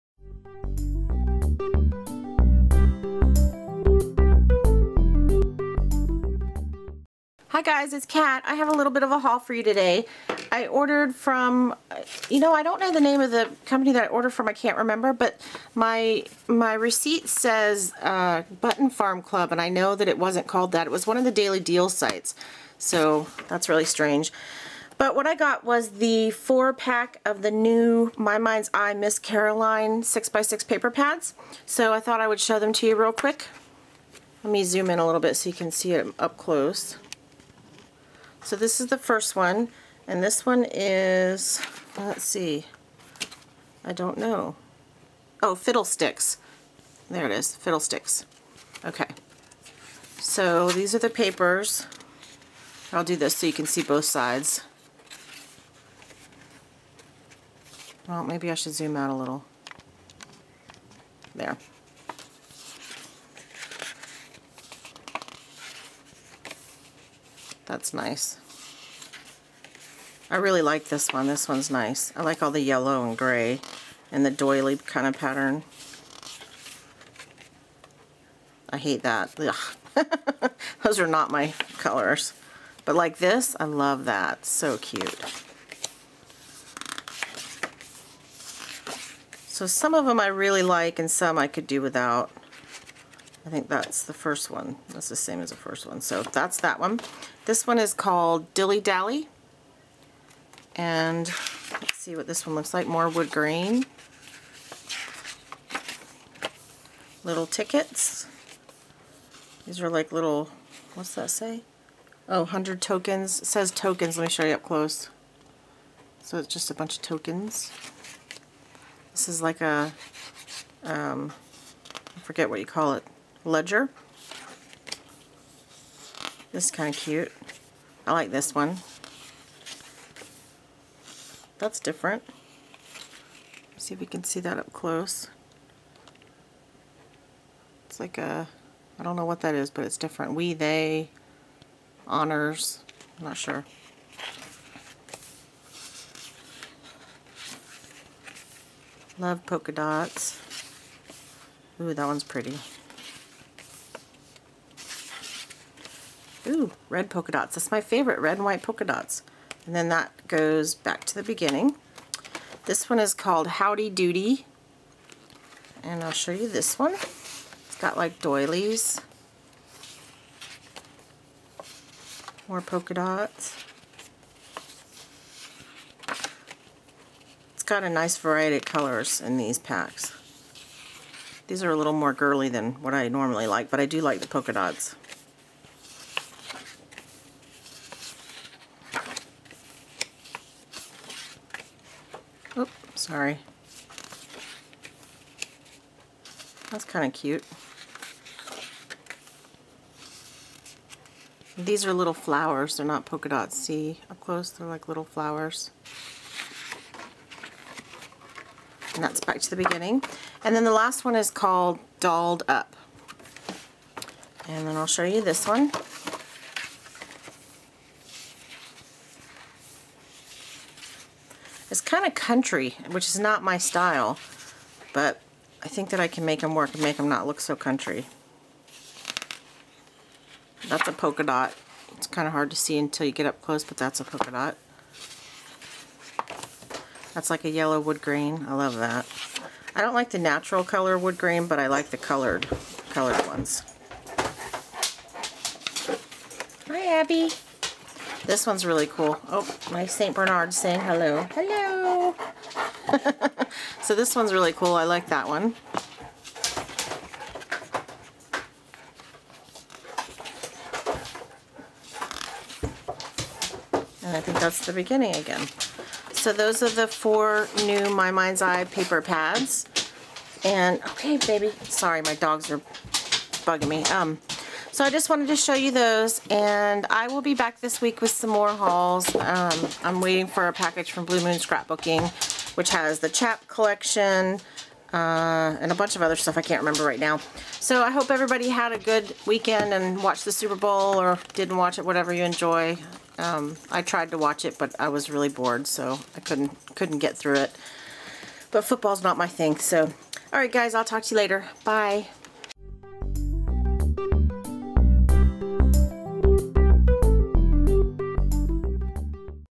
Scrapbooking Haul
Scrapbooking Haul - New 6x6 Paper pads from My Mind's Eye (Miss Caroline)